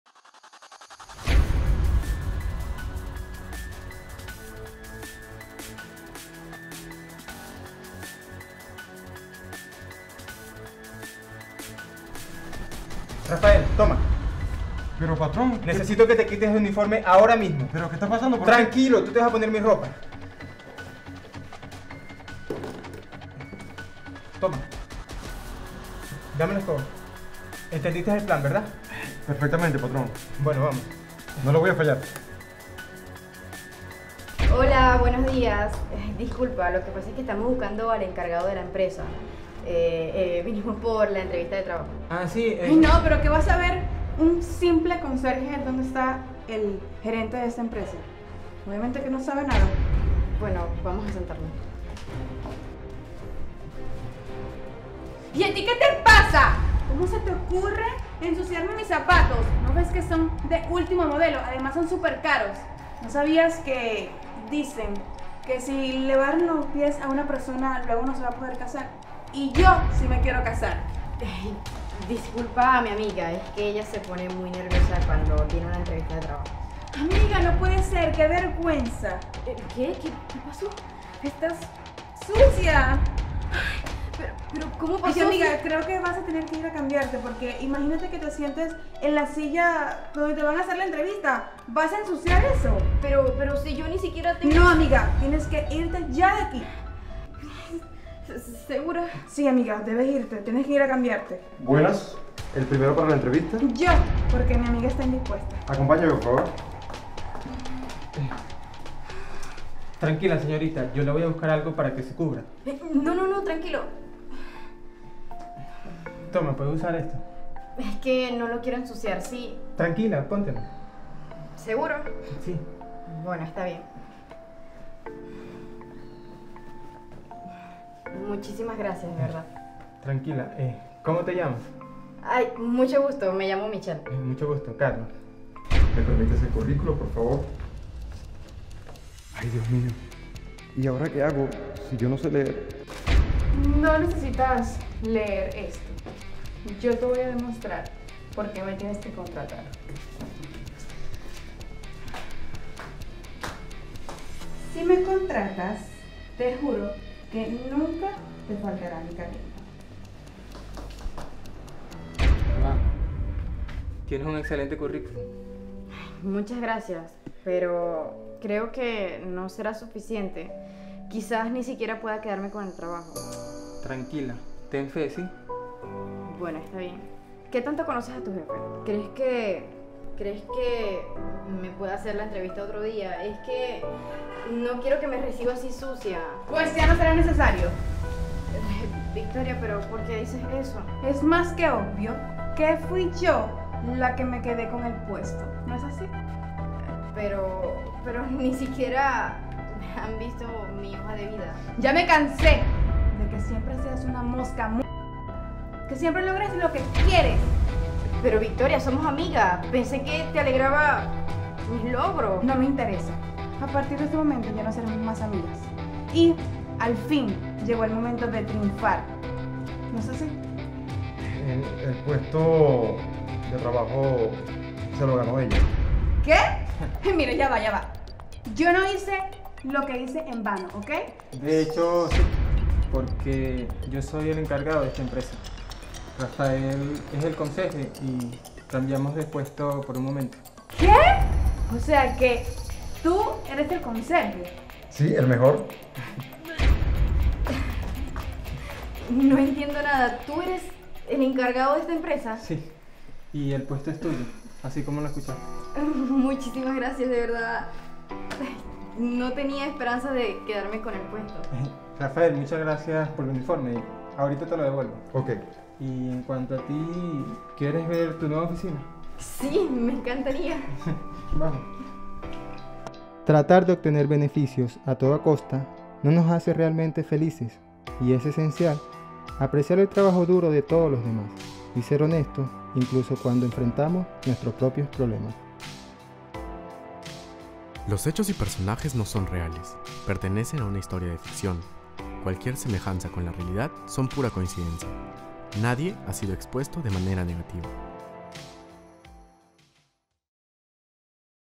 Amiga envidiosa le tira mostaza a su amiga | Envidiosa arruinó la entrevista de trabajo de su amiga
Esta envidiosa arruinó la entrevista de trabajo de su mejor amiga. Amiga envidiosa le tiró mostaza antes de la entrevista de trabajo. El envidioso no quiere lo que tú tienes, lo que quiere es que tú lo pierdas.

Policía planeaba sorprender a su esposa y ella fue a tatuarse el nombre de su amante